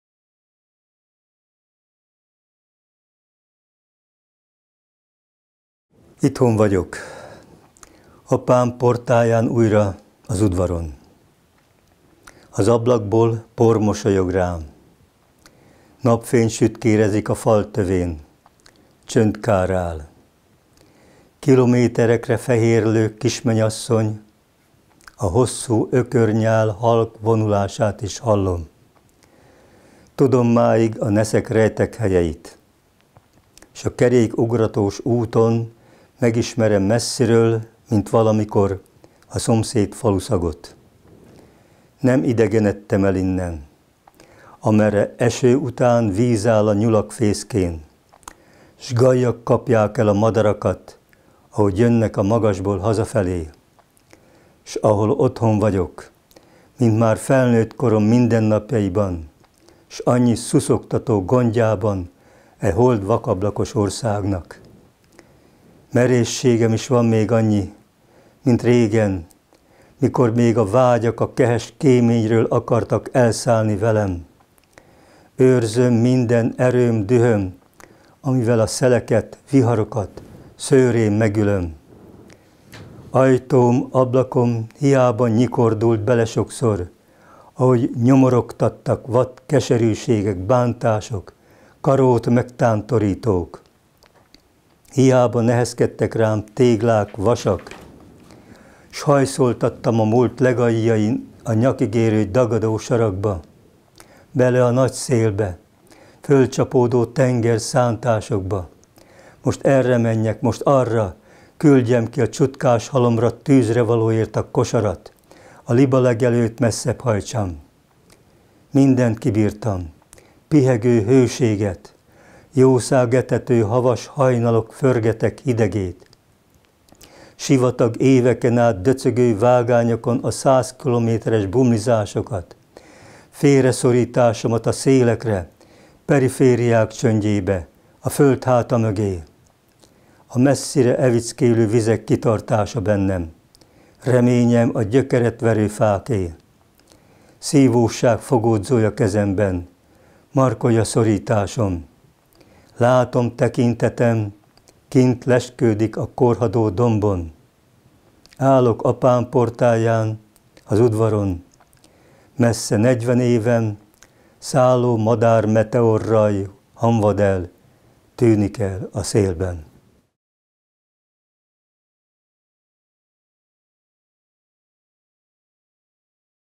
Serfőző Simon: Őrzöm - a költő előadásában
A felvétel 2015. januárjában készült Zagyvarékason.
MMA, 2016